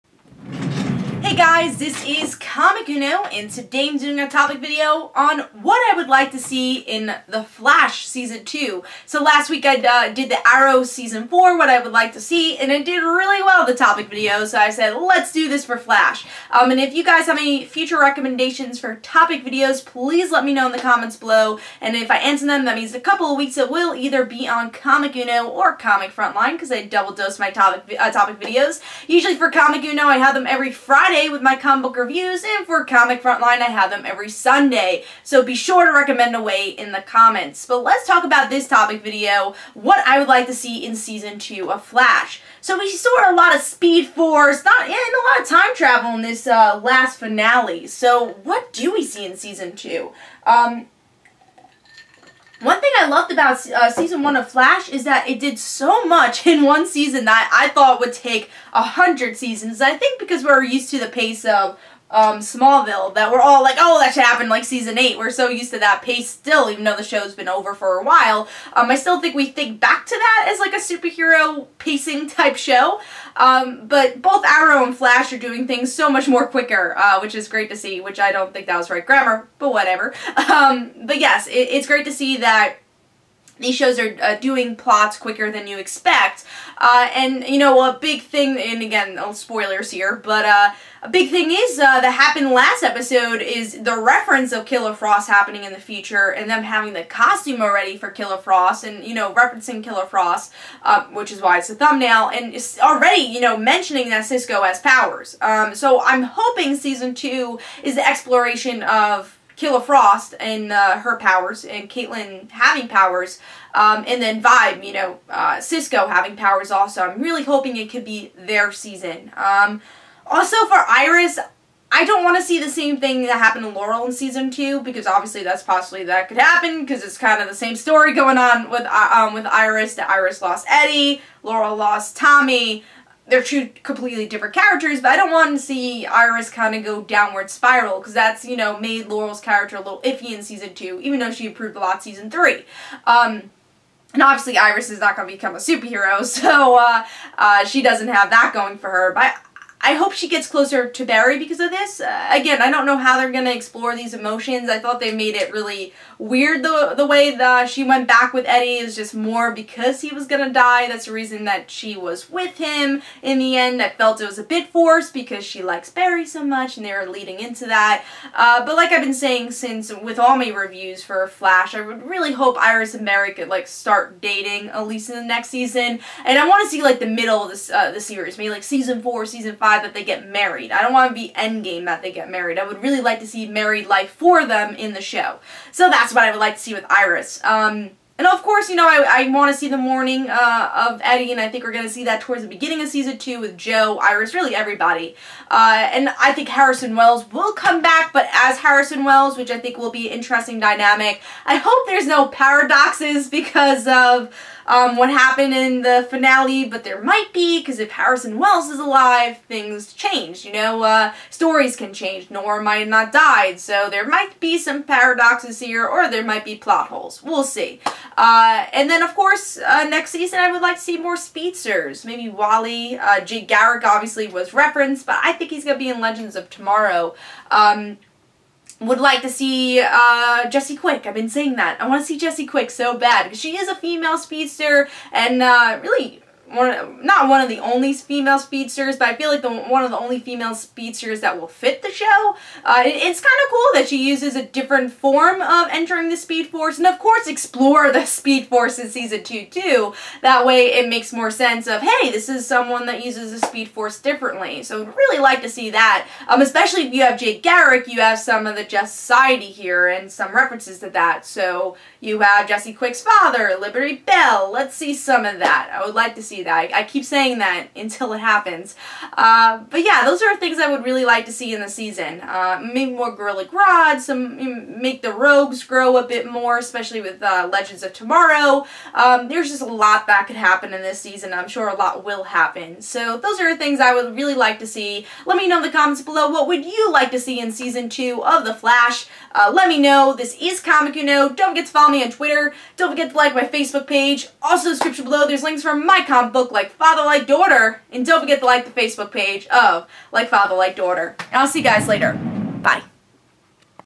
Comic Uno What I want to see in Season 2 of Flash? (Topic Video)
Here is my topic video What I want to see in Season 2 of Flash.